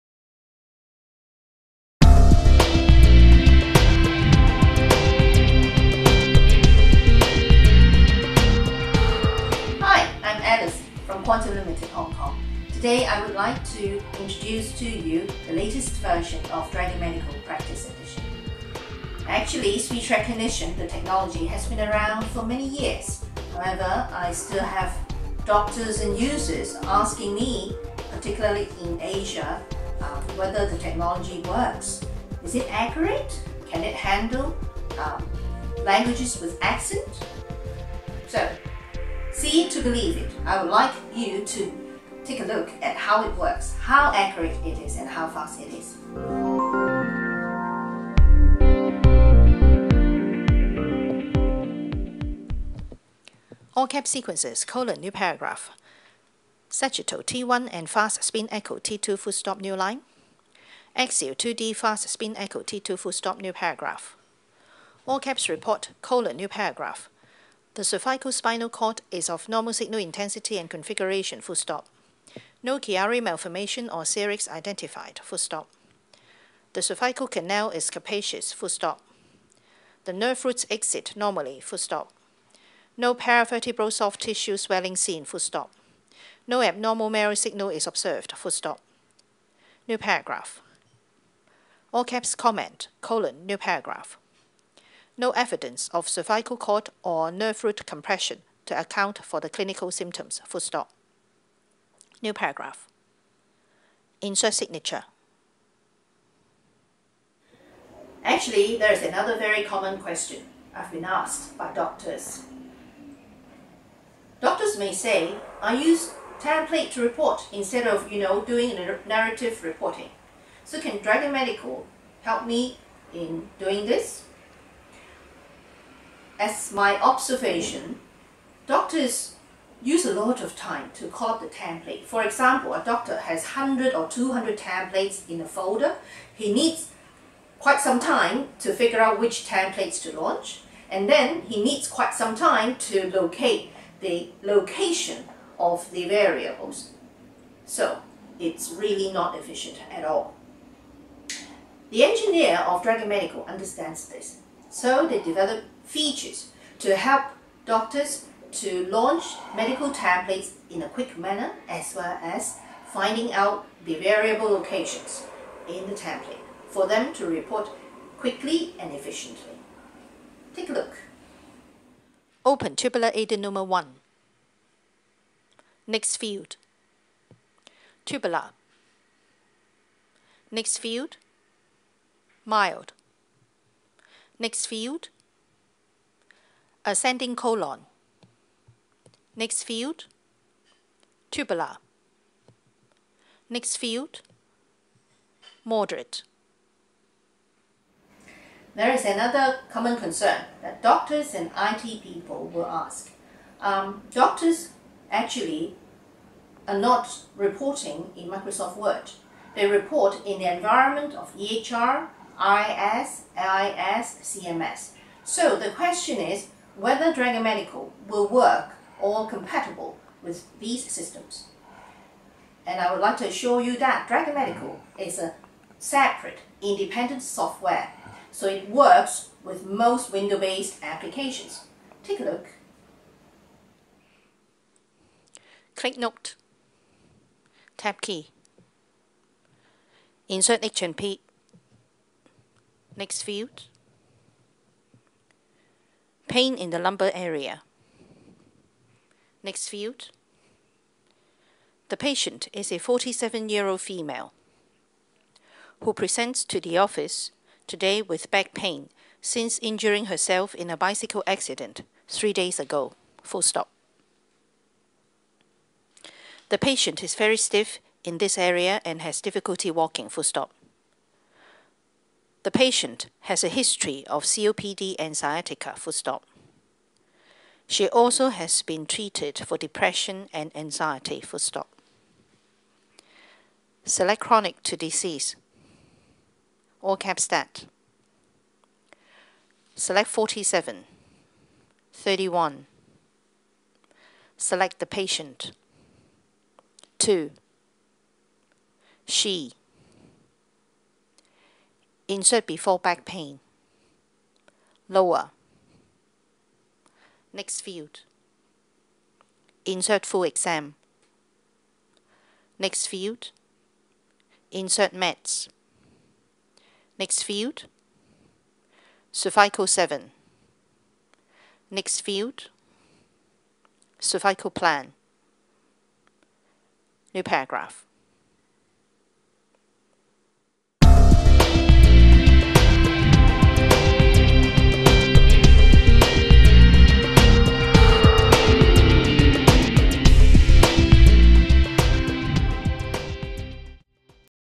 Seeing is Believing - the Power of Dragon Medical explained
Dragon Medical Practice Edition 2 demo by certified trainer Alice.  The demo consists of 3 parts.  Part one is a demo of a radiology report.  Part 2 is dictating a pathology report using template.  And the third part is to demonstrate compatibility with third party reporting programs such as EHR, PACS, HIS, etc.  Contact us to try Dragon with your own voice and watch the words appear right before your eyes on your own PC.

Dragon Medical Practice Edition 2 is:

20% more accurate than the previous version
Dramatically faster recognition speed
Over 30 new specialties, totaling over 90
Regional accent support
Improved Hidden Mode makes dictation into an EMR a snap!